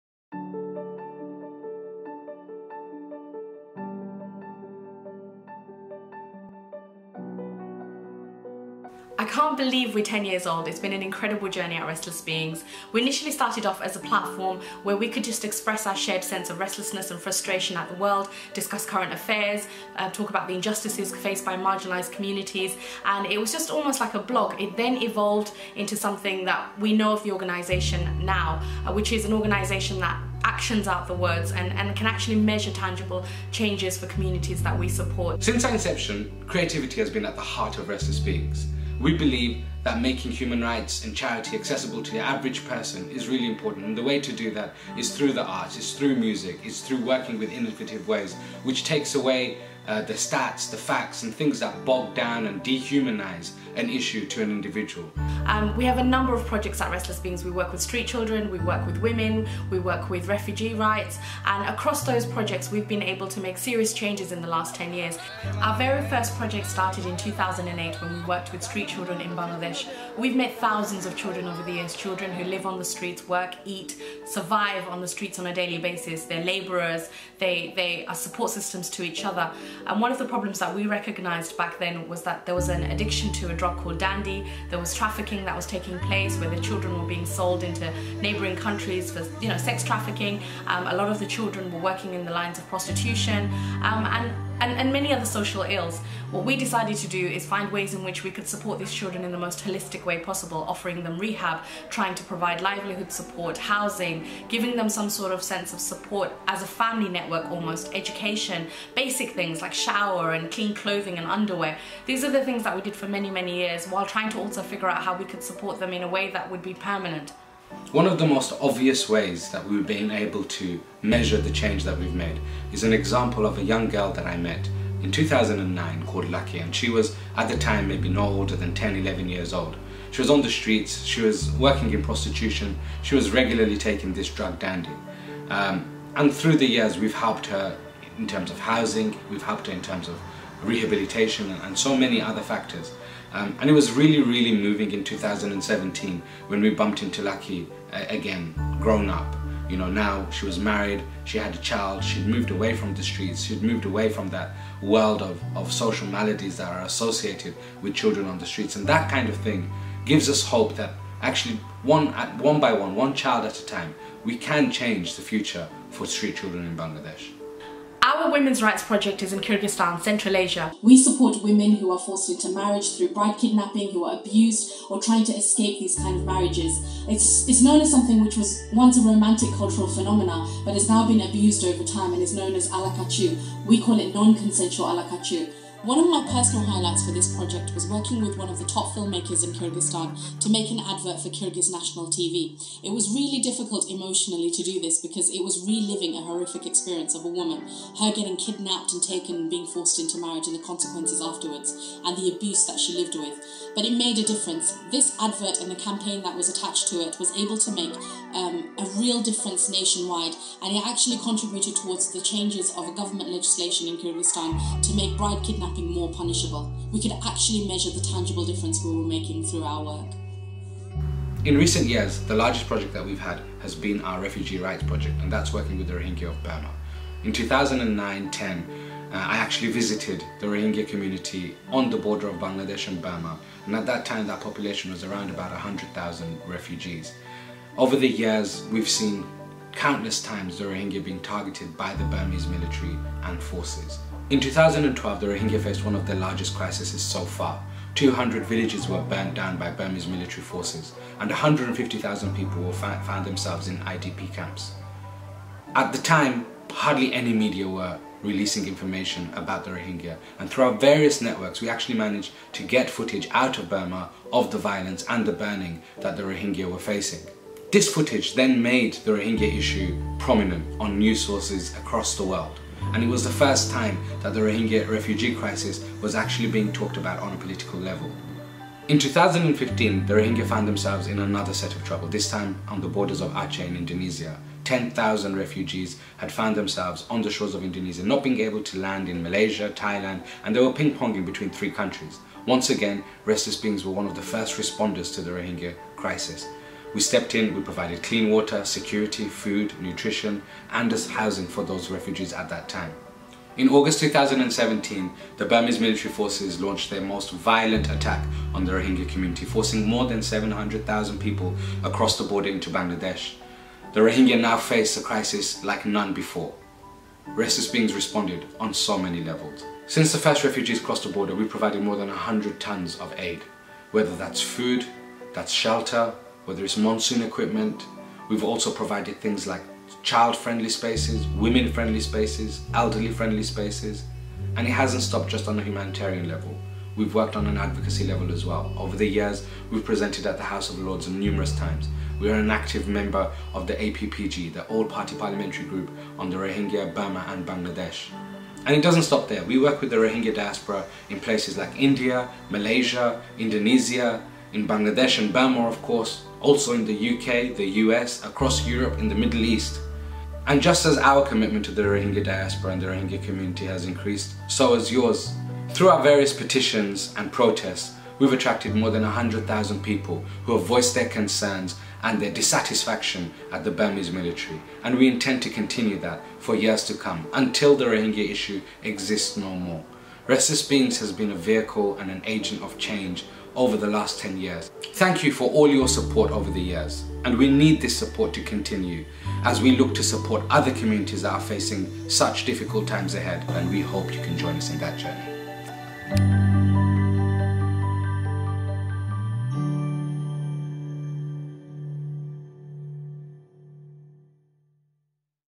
A Decade of Activism, Advocacy and Charity
Restless Beings came into fruition in 2008. This video looks back at the last 10 years of our work - from child rights for street children in Bangladesh, to women's rights in Central Asia (bride abduction) to refugee rights and our work with the Rohingya of Burma.